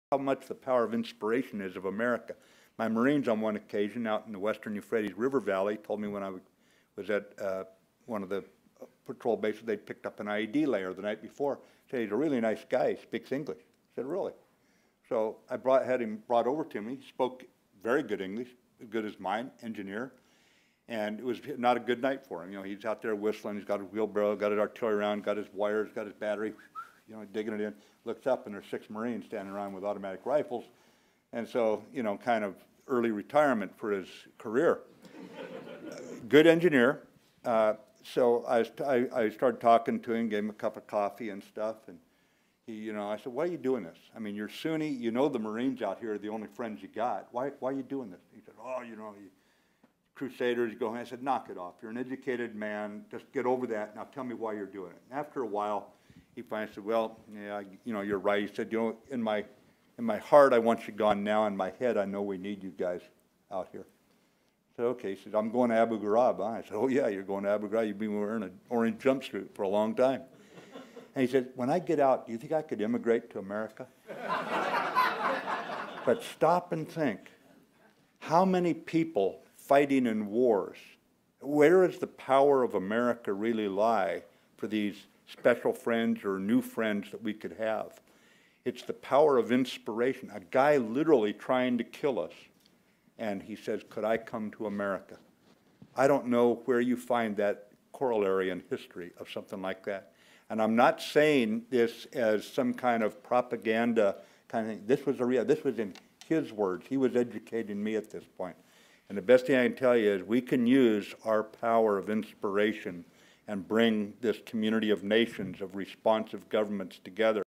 The Inspirational Power of America: General Mattis Shares Amusing Story About A Terrorist Engineer
Video excerpt courtesy 'The Heritage Foundation' (from source freely available on YouTube)